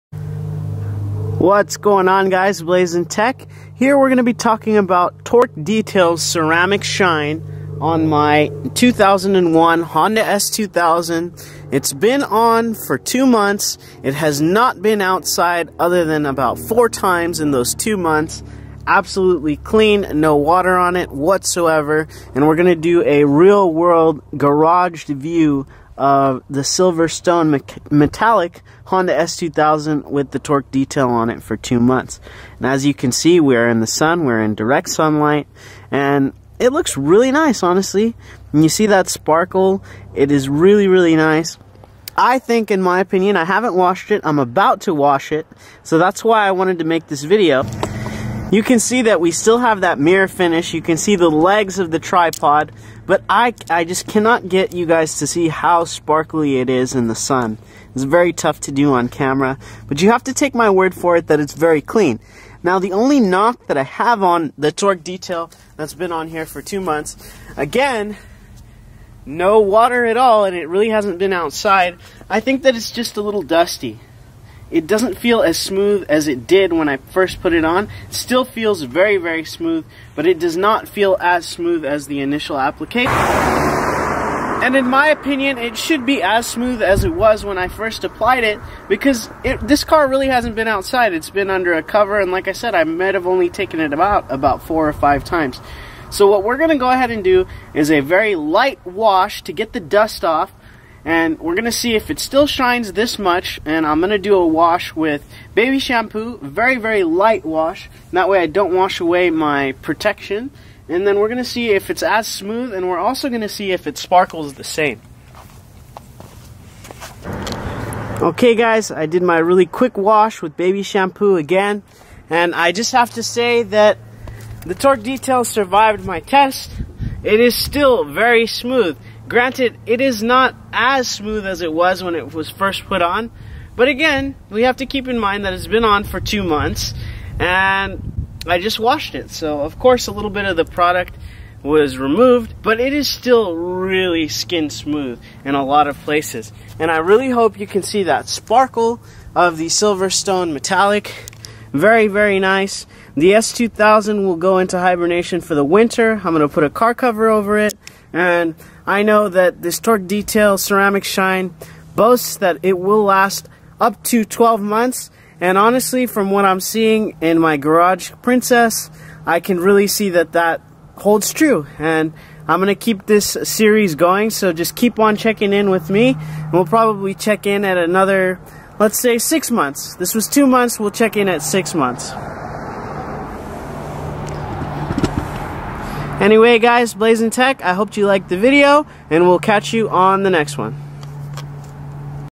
Torque Detail Ceramic Shine Garaged Car: 2 Month Update
Hello all, today I am testing torque detail's ceramic shine on my Garaged Honda S2000. This product boasts 12 month protection and we are going to put it to the test! check in for a 6 month update!